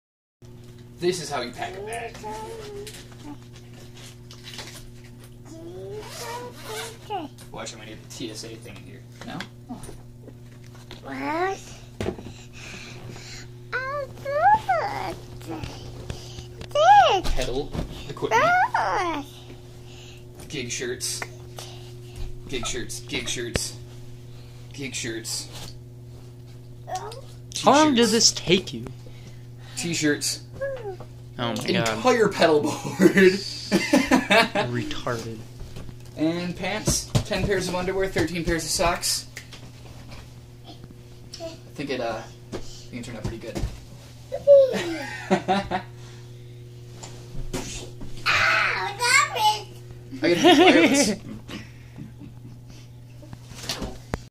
This is how you pack a bag!
I had to bring all of my pedals to Cleveland with me... This is how I did it!

Travel Rig:

EHX POG2
Digitech JamMan Delay
Shure Wireless
VOX Clyde McCoy Wah
TC Electronics Corona Chorus
TC Electronics Hall Of Fame Reverb
ZVEX Fuzz Factory
Boss DS-1 Distortion
Xotic EP Boost